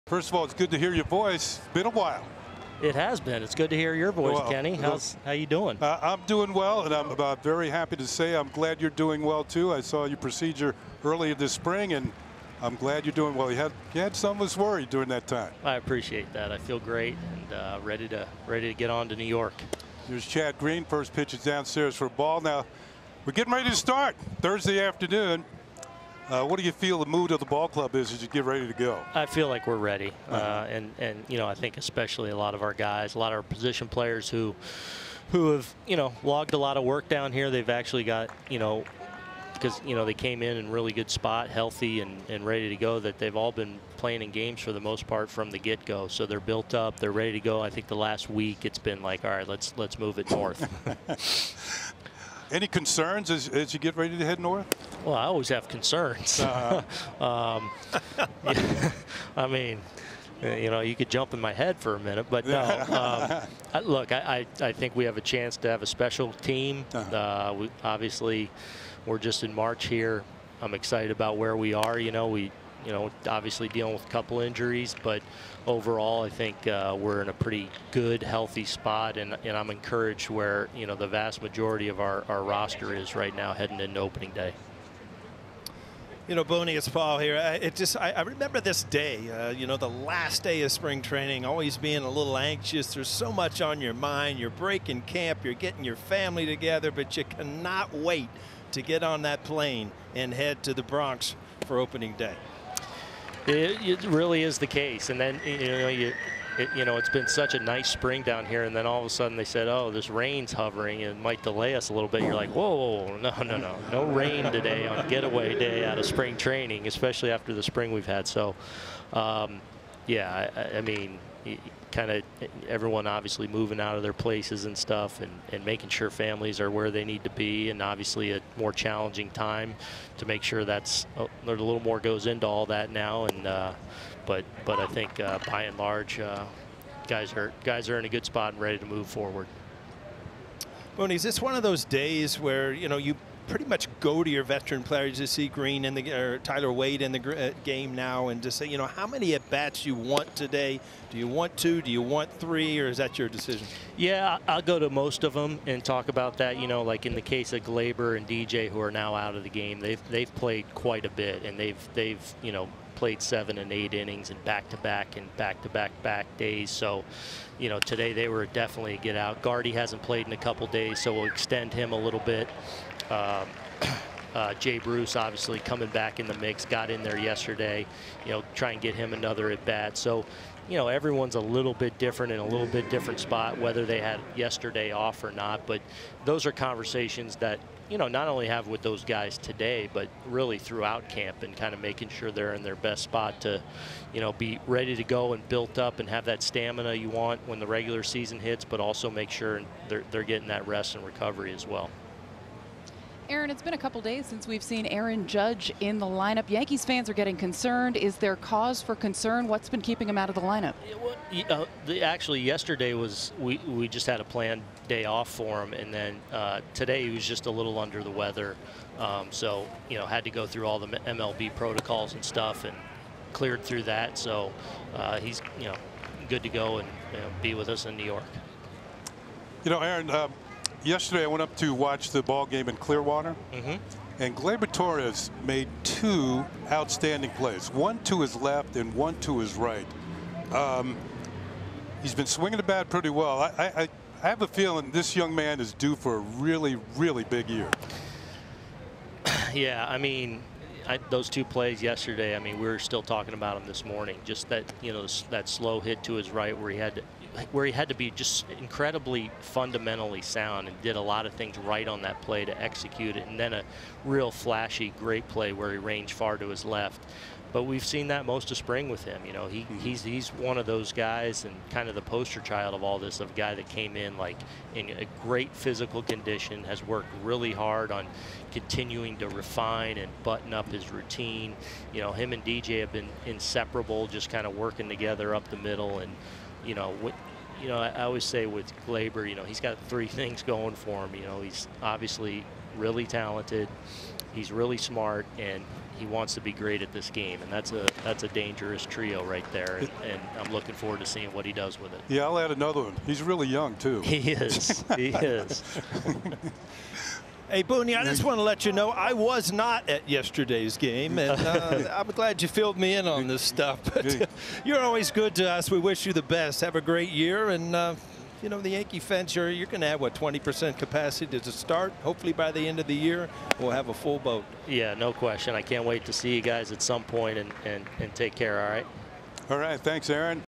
Aaron Boone with updates during the final game in camp
Yankees manager Aaron Boone with updates during the final game in Spring Training as the club gets ready for Opening Day.